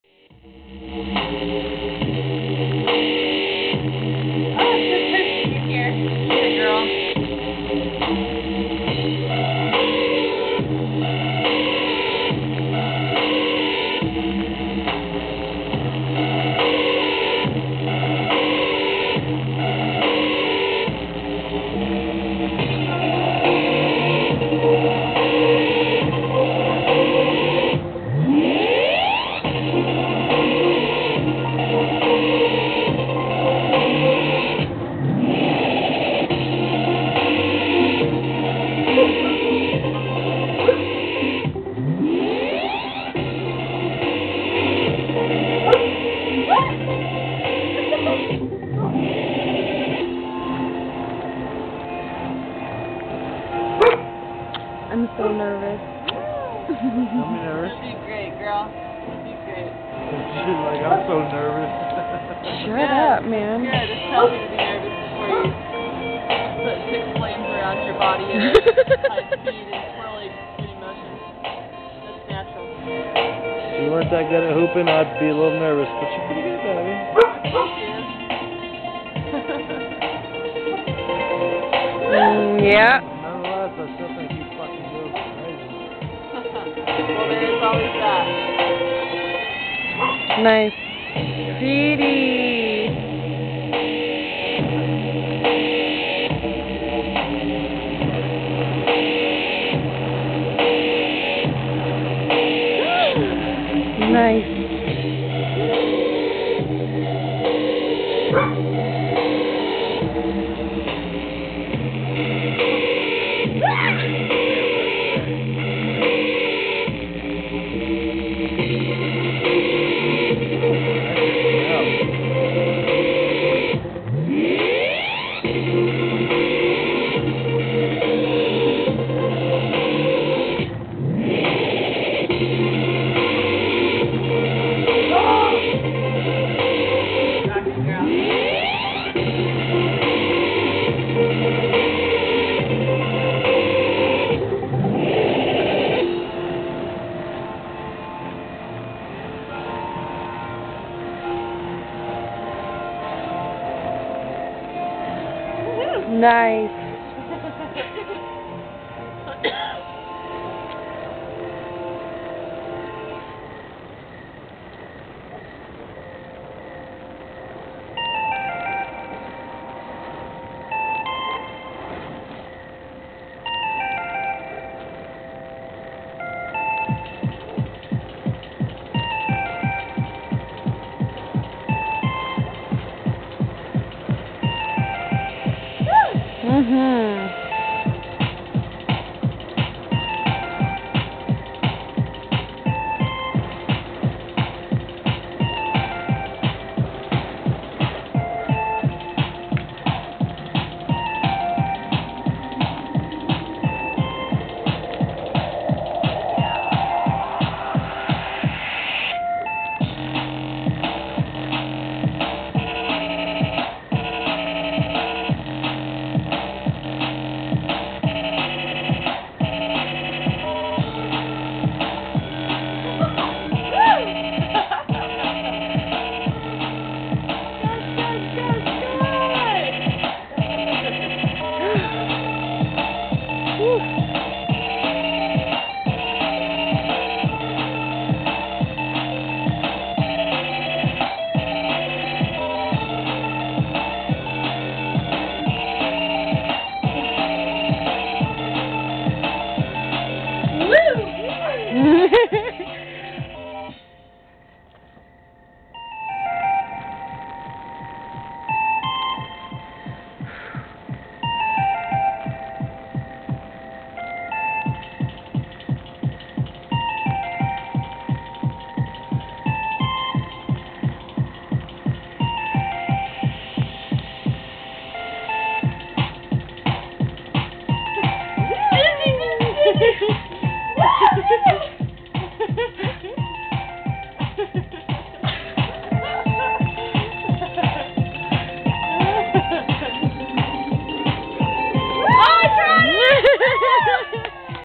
Cally's Burn
My friend Cally, rockin' the heck out of that fire hoop!!